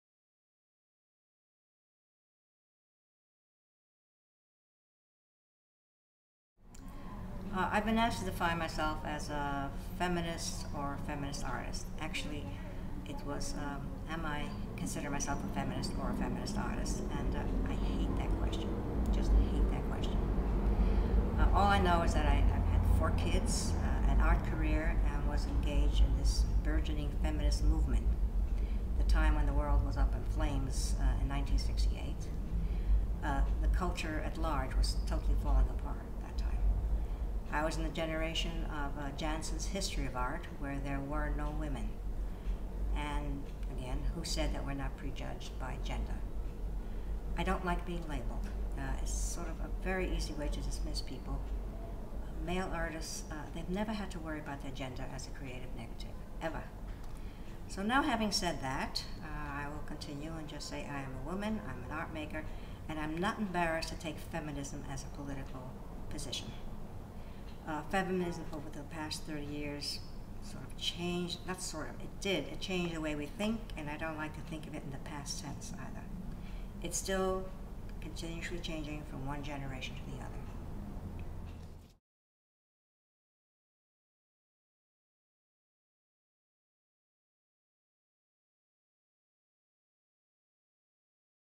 Ida Applebroog
Burning Down the House: Building a Feminist Art Collection is an exhibition of nearly fifty works focusing on recent acquisitions and major loans, including works by artists such as Kiki Smith, Tracey Emin, Tracey Moffatt, and Lorna Simpson.

Burning Down the House
October 31, 2008 - April 5, 2009
Brooklyn Museum